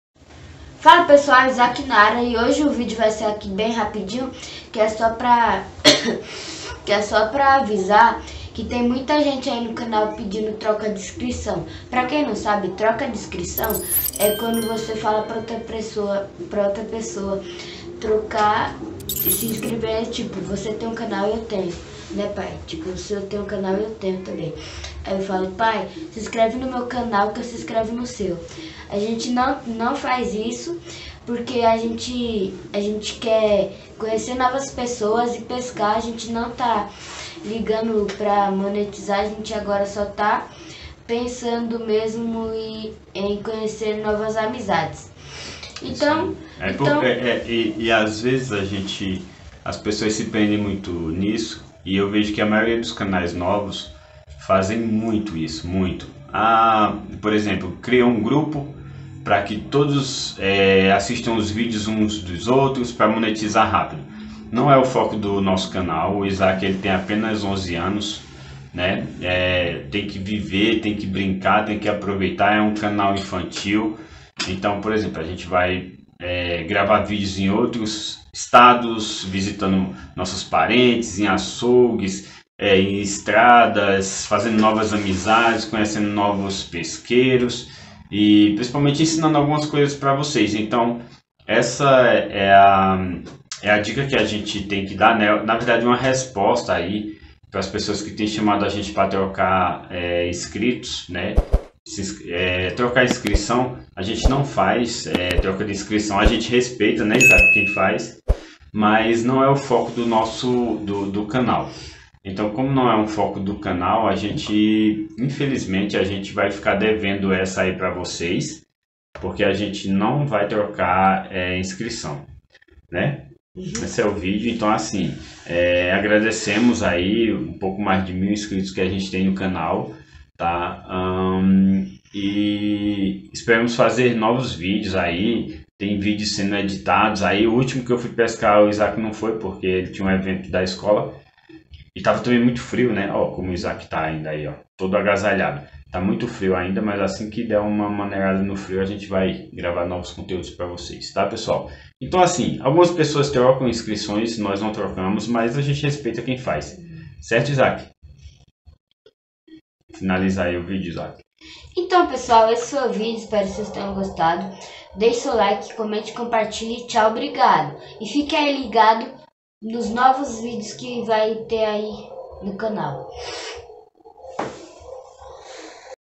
NUNCA FAREI ISSO NO MEU CANAL.
Nosso canal é para guardar recordações com amigos, lugares que estamos conhecendo e lembranças com nossa familia.

Por isso nunca farei isso no meu canaç de trocar inscrições.